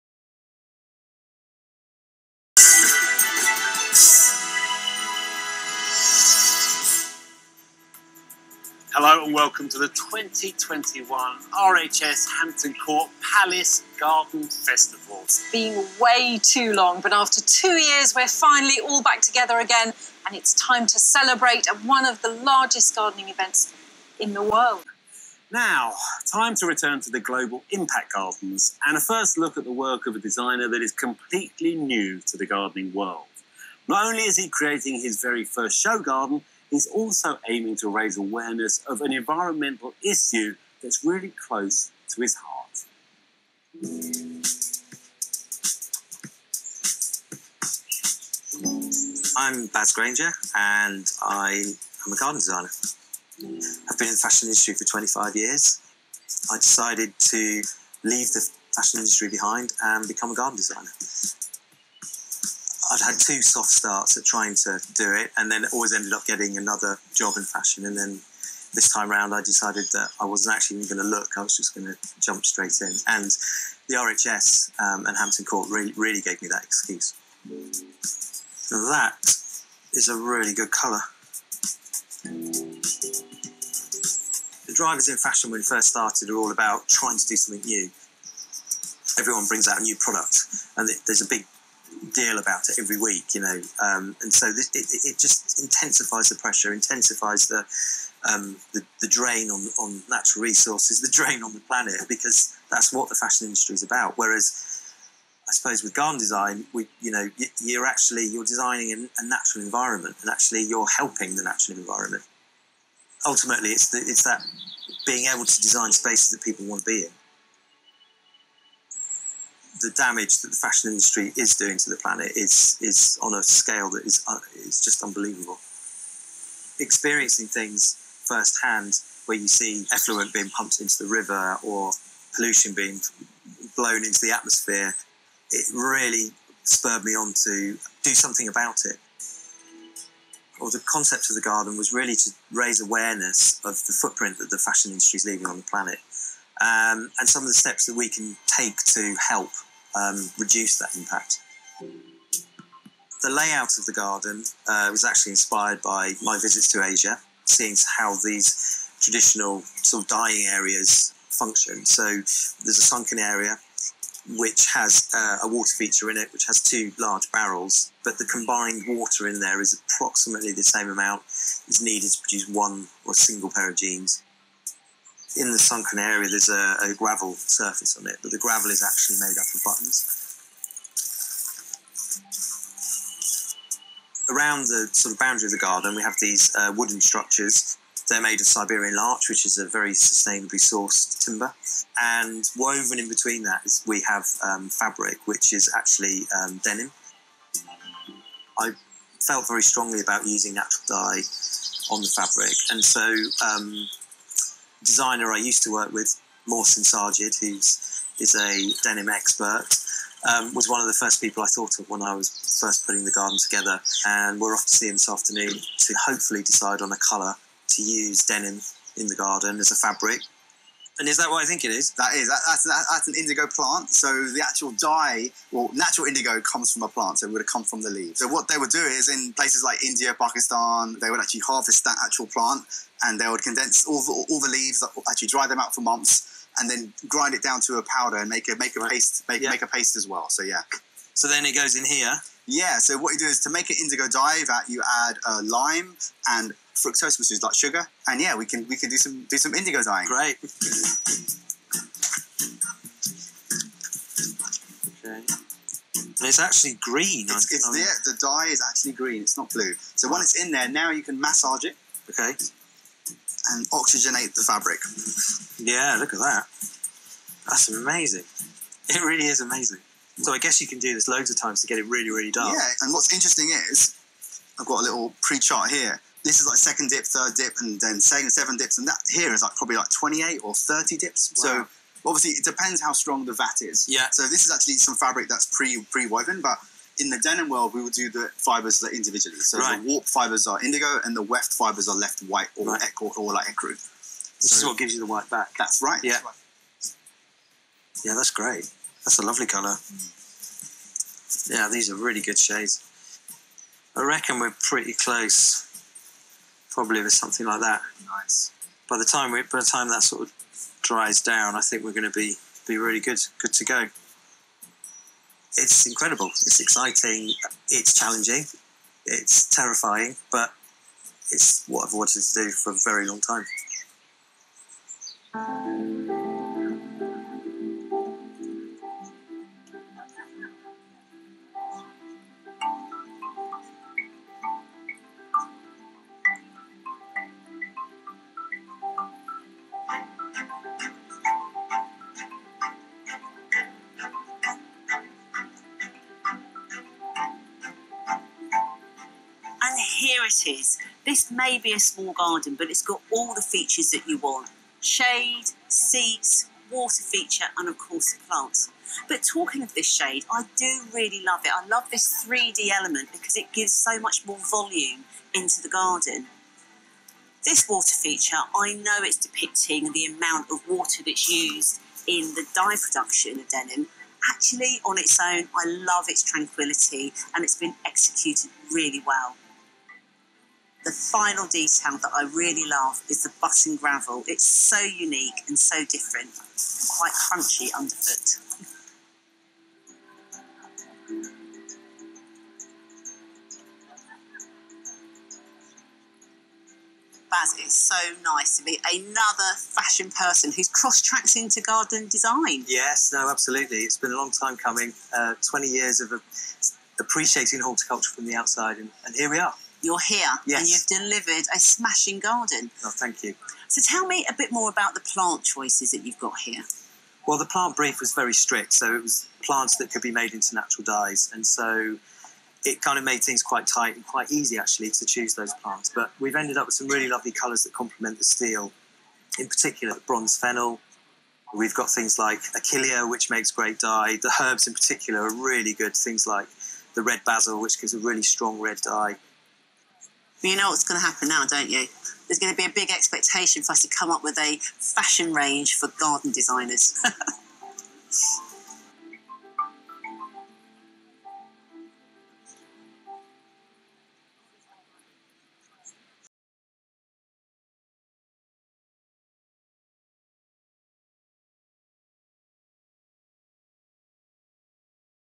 RHS Hampton Court Palace Garden Festival 2021 - The Landscape Tailor - Fashion Footprint Garden
RHS Hampton Court Palace Garden Festival 2021 -
The Fashion Footprint Garden

Designed by Baz Grainger - The Landscape Tailor

CONE DENIM®
ENDRIME®
Totally Buttons

Baz Grainger - The Landscape Tailor, won Silver Medal and Best Global Impact Garden in 2021 RHS Hampton Court Palace Garden Festival 2021.

Here is the footage captured on BBC on 7th July 2021.

Barry used CONE DENIM® for is garden design and worked with Mohsin Sajid from Denim Consultancy ENDRIME® on use of fabrics and Special Aprons for the staff at the show..

A mixture of 2 CONE DENIM® fabrics were used -

Westerly # J58971, 11.5oz 3x1 RHT Denim
Cottonwood # N008J, 9.75oz 3X1 RHT Denim

In the short Film, we follow Baz in his home studio designing the garden, then he visits ENDRIME® and meets with Mohsin, who shows him natural indigo dying, then we end with Baz at Hampton Court Palace Garden Festival talking with Arit Anderson.

The Fashion Footprint Garden details:

▪ The dramatic zig-zag screening at the back of the space is made from environmentally responsible materials: sustainable timber framing is entwined with recycled fibre fabrics

▪ Two round Corten-steel barrels are reminiscent of the vats that are used in the indigo-dyeing process in India and Asia; interestingly, the amount of water here represents the amount used to make a single pair of jeans. Planting abounds with flowering echinaceas, achilleas and silver birch trees
About the garden

The Fashion Footprint Garden focuses on the impact the fashion and textile industries have on the environment and some of the steps being taken to mitigate the impact. The garden takes inspiration from the textile dyeing pits seen in India and Asia. The sunken area contains a water feature with two Corten-steel barrels reminiscent of the dye vats used in the indigo-dyeing process. The volume of water here equates to that used to make a single pair of jeans.The garden incorporates textile waste in the form of gabions filled with old clothes. Buttons are used for gravel. The boundary fences are made from sustainable timber woven with fabric made from recycled fibres. The planting is naturalistic and colourful and softens the hard edges of the Corten-steel planters. All the herbaceous plants can be used to make natural dyes, which help reduce water pollution in the industry. The trees and shrubs are species that are highly efficient in filtering air pollution, such as the silver birch trees and yew domes.

With special thanks to Kara and Pierette at Cone Denim, Mohsin + Sadia from Endrime and Totally Buttons.